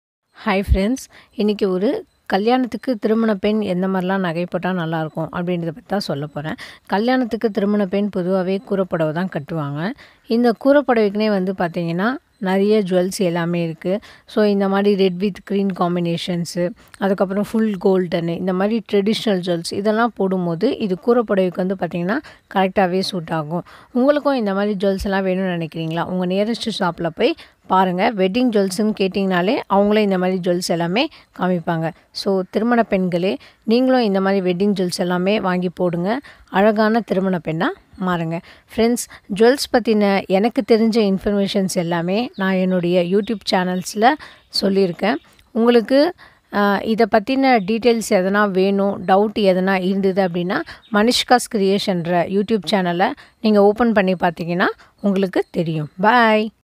Thirumana covering nagai is the Best for Koorai Pudavai
For Kalyana Koorai  Pudavai covering jewels are the best than gold if you look at red with green and full gold color it suits best for the best. So wear Covering and glitter as stars.